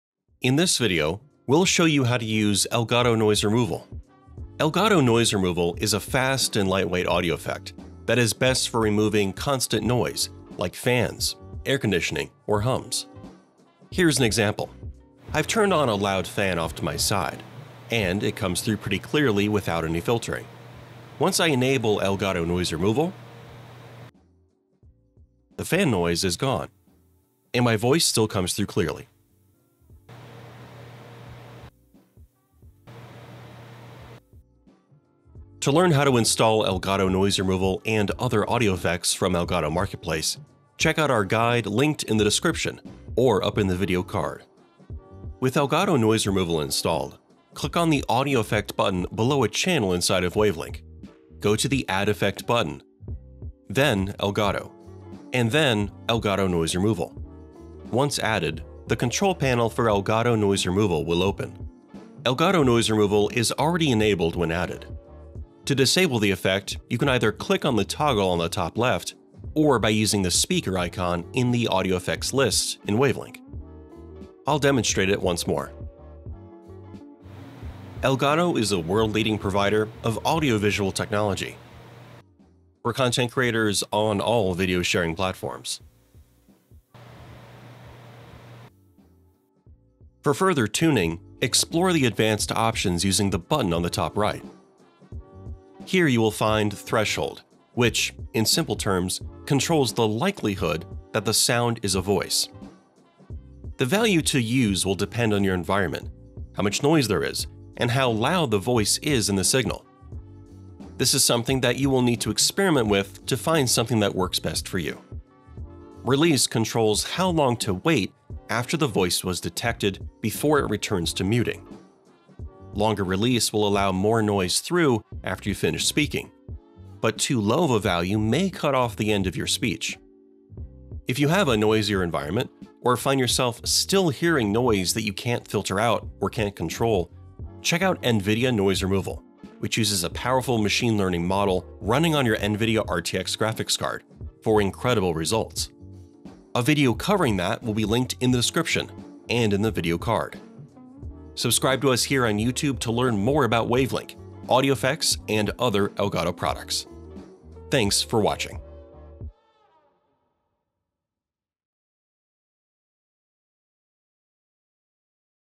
How to Use Elgato Noise Removal to Clean Up Your Sound
Silence background audio with a single toggle. Enhanced by AI technology, this effect filters out distracting noise like fans, air conditioners, and PC hum. It’s ultra lightweight, works with almost any CPU, and requires no special GPU hardware. Try Elgato Noise Removal in Wave Link and sound crystal clear — no matter your environment.

00:00 - Intro
00:45 - Adding Elgato Noise Removal to a Wave Link channel
01:05 - Controlling Elgato Noise Removal
01:30 - Advanced options